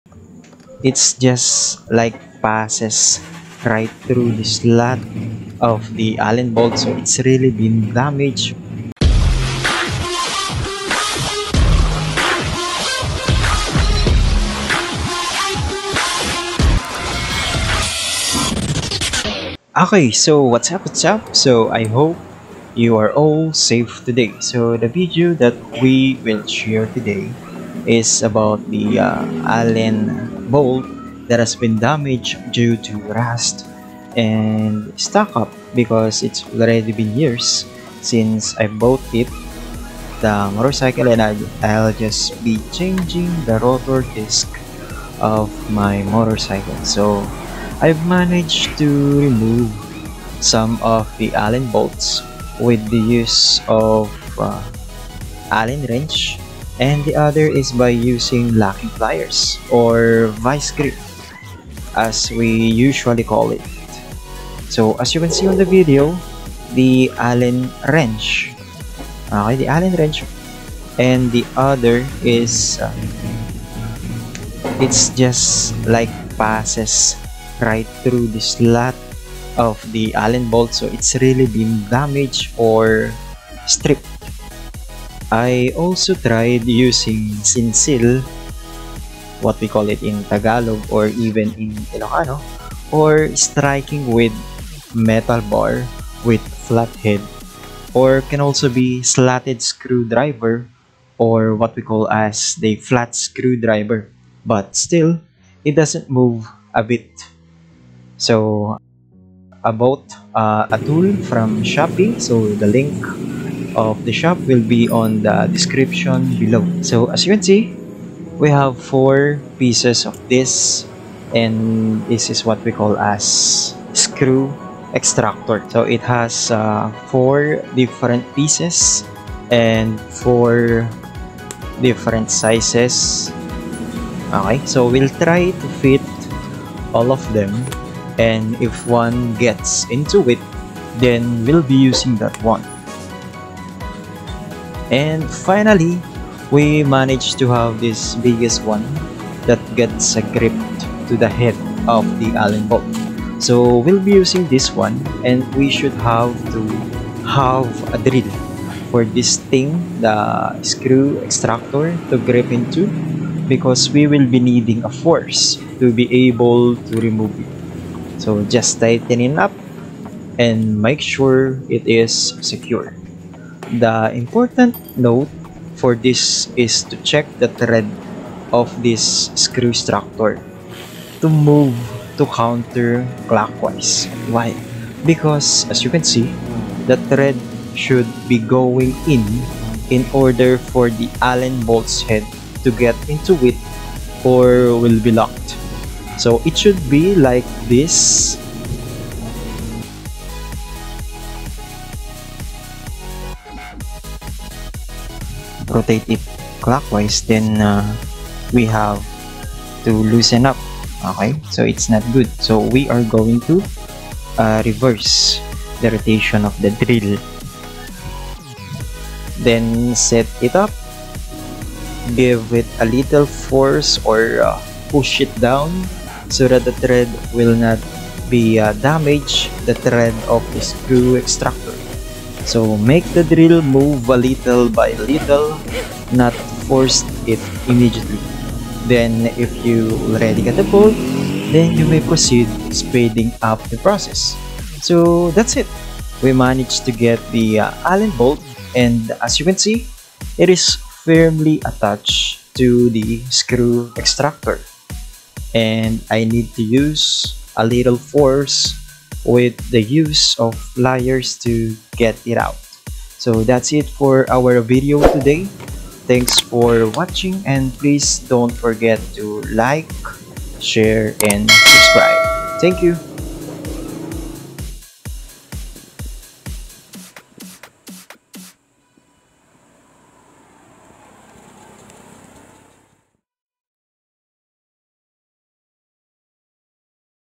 How to remove damaged bolt / screw / (paano tanggalin ang screw) by using Damaged screw extractor
video shows one way to remove damaged or stripped allen bolt